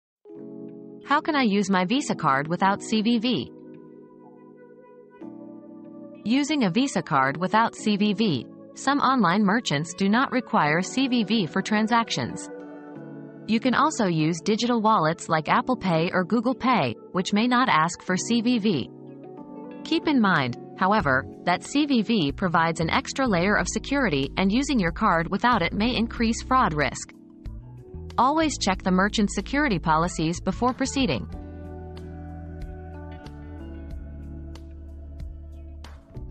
How can I use my Visa card without CVV?
Unlocking Visa Card Transactions Without CVV • Visa Without CVV • Discover how to use your Visa card without needing to provide a CVV code. Learn about online merchants and digital wallets that allow for CVV-free transactions, but be aware of potential security risks.